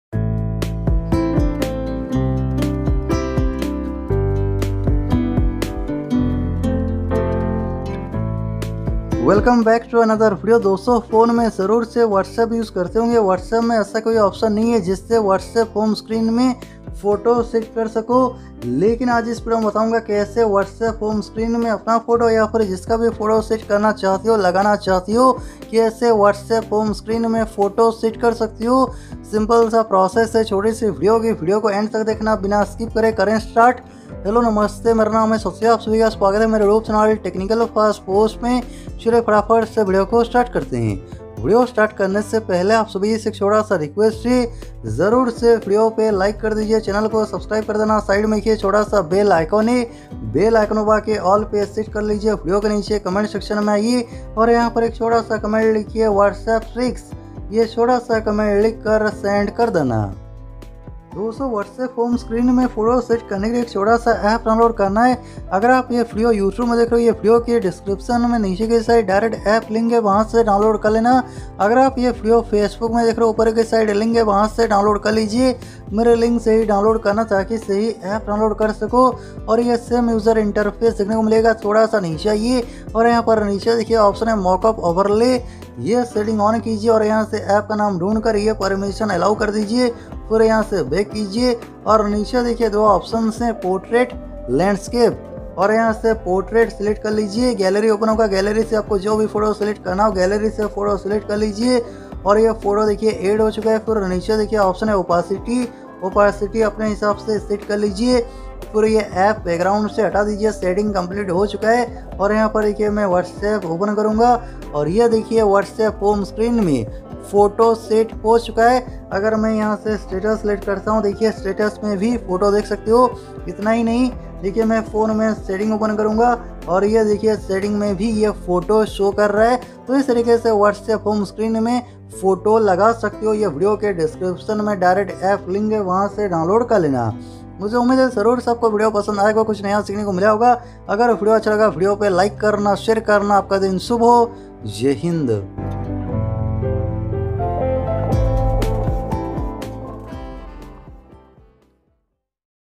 WhatsApp के Homescreen पे अपनी फोटो लगाए! How To Apply Wallpaper in Whatsapp Home Screen | 2023
How To Apply Wallpaper in Whatsapp Home Screen | WhatsApp के Homescreen पे अपनी फोटो लगाए! Technical Firstpost

Namaskaar Dosto, es video maine apko bataya hai ki apne whatsapp homescreen maine apni photo kaise laga sakte hai. Mujhe umeed hai apko video pasand aayegi.

in this video i will show you how to change WhatsApp home screen background.

🔴HASHTAGS USED:-

🔴QUERIES SOLVED:-